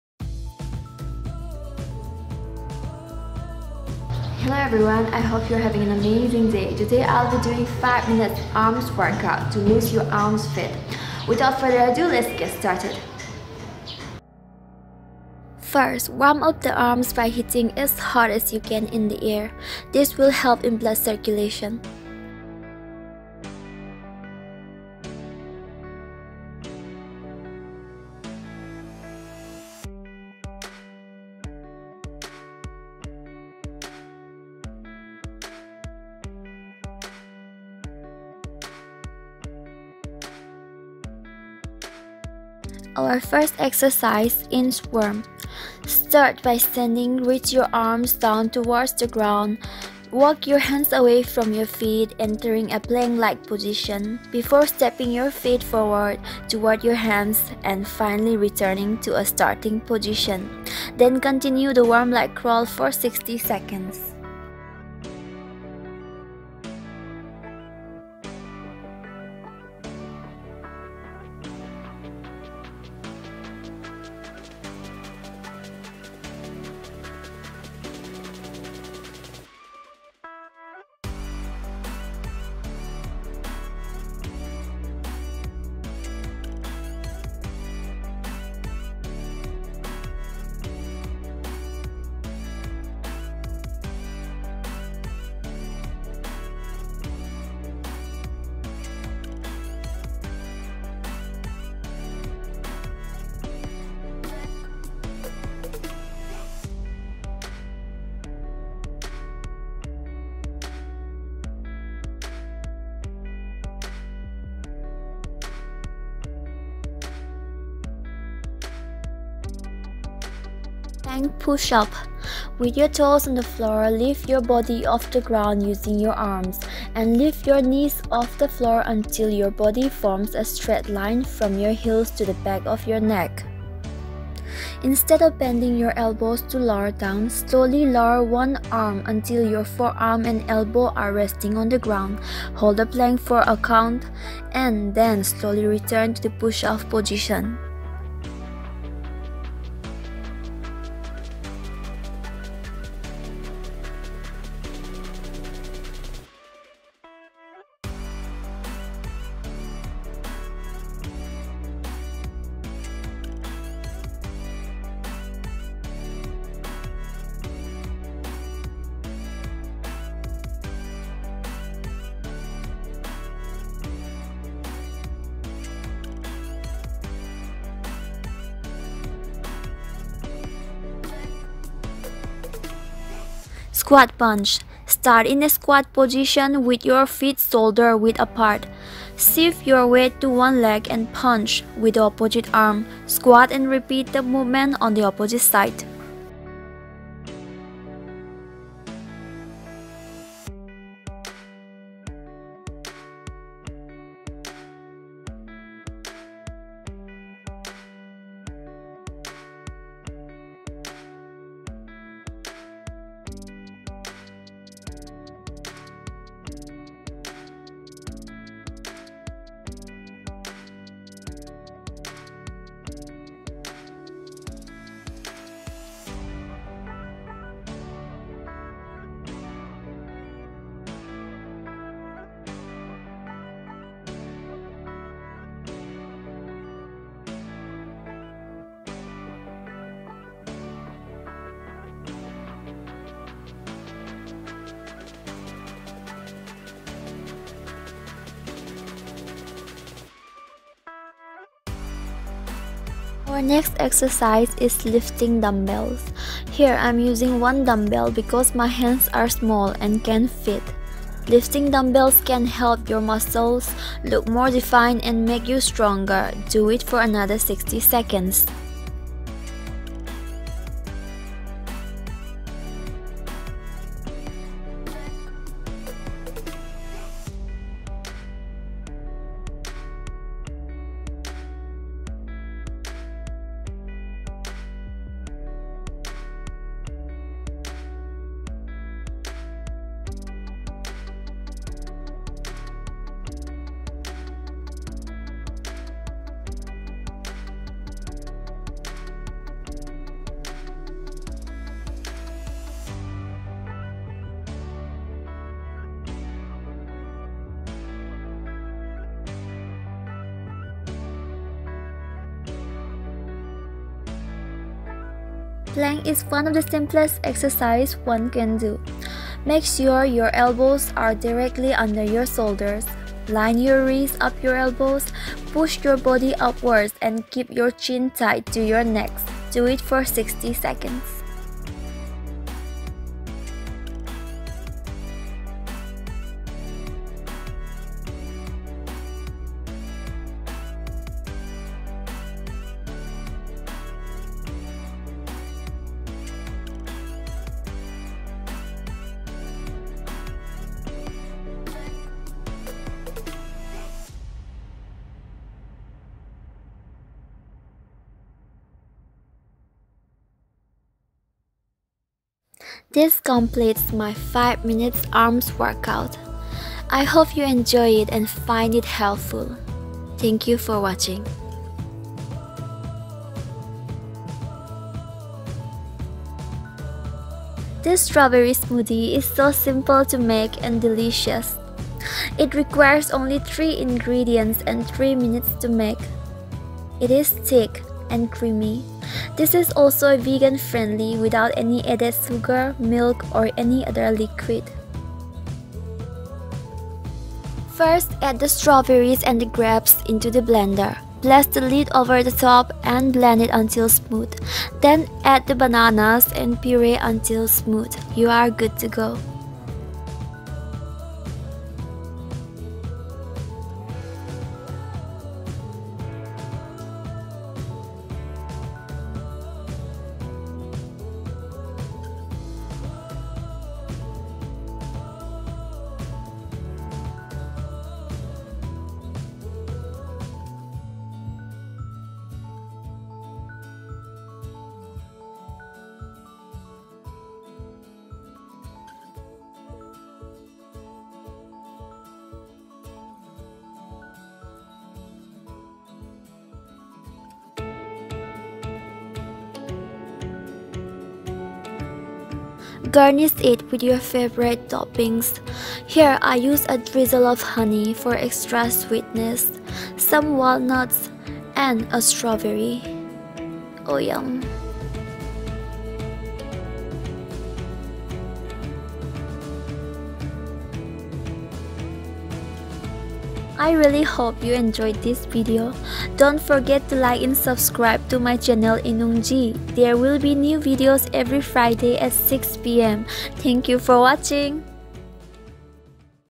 5 Minutes arms workout and Strawberry banana smoothie
Hey there

I'm back with an easy but intense arm workout. It may seems like it is going to be easy but hold that thought right there. Although this arm workout is short it is very effective and tones your arms muscles.

There is also an easy and quick vegan friendly strawberry banana smoothie recipe. I hope you enjoy it.🌸🌸🌸

Thanks.